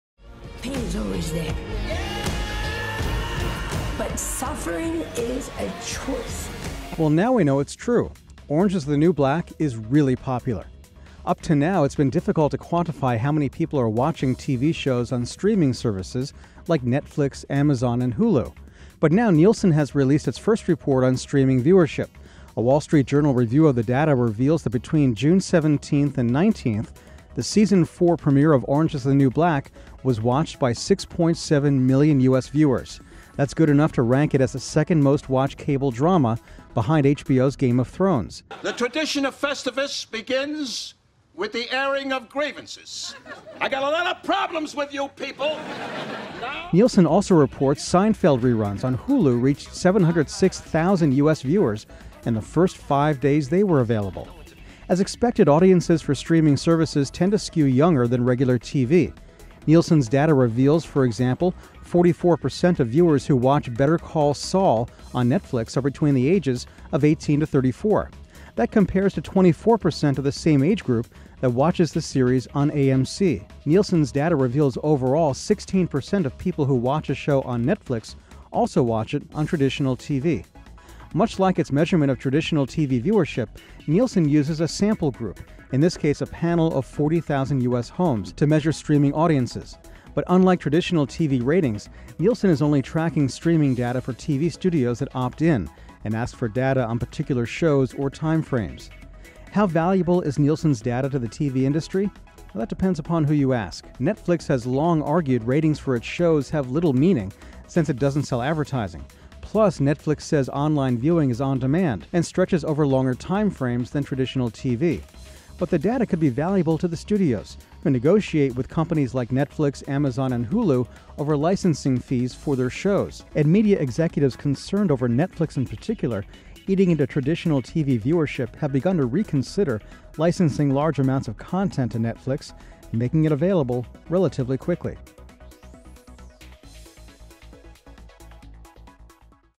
Nielsen: 'Orange Is the New Black' Is a Ratings Hit
For the first time, Nielsen has released ratings for TV shows on streaming services.  The new data confirms the popularity of Netflix's "Orange Is the New Black," and could provide other valuable insights to studios. Photo: Netflix